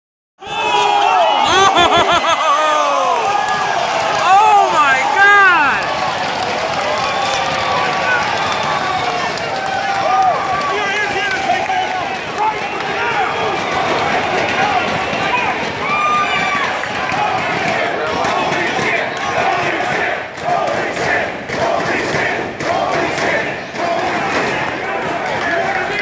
Keith Lee's Corkscrew Plancha
Keith Lee showing 320 lb agility in Amarillo, TX. Ring of Honor 5/29/15